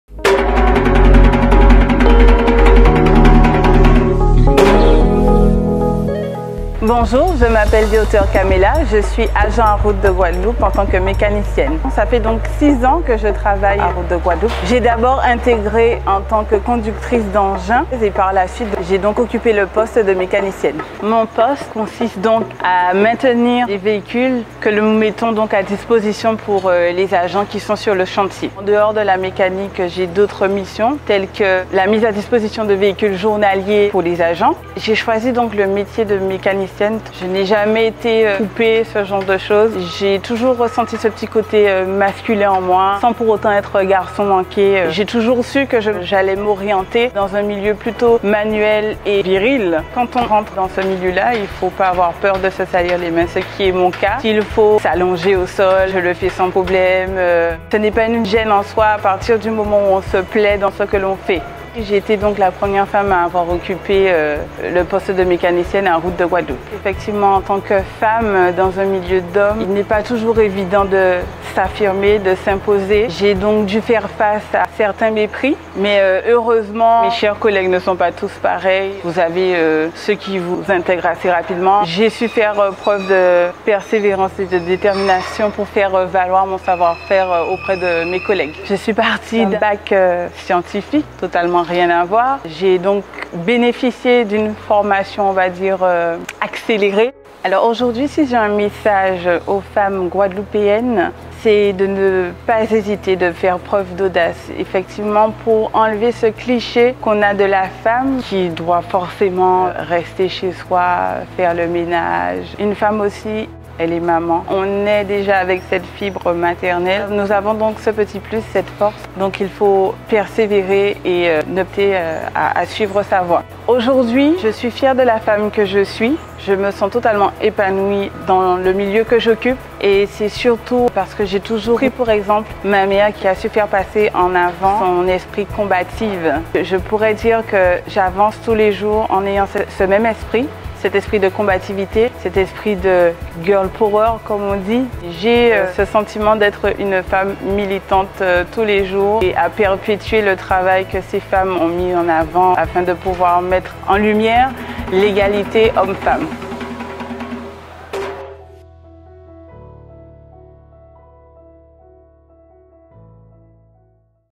[Archipel d'Avenir] - Kaméla DESHAUTEURS, Mécanicienne à Routes de Guadeloupe
𝐀𝐫𝐜𝐡𝐢𝐩𝐞𝐥 𝐝’𝐀𝐯𝐞𝐧𝐢𝐫 | Pour cette 1ère édition de la série « Archipel d’Avenir » découvrez les portraits des femmes qui œuvrent au sein de la Région Guadeloupe.

Aujourd’hui, faites connaissance avec Kaméla DESHAUTEURS, Mécanicienne à Routes de Guadeloupe.

Fanm Gwadloup, nou ka di zòt mèsi !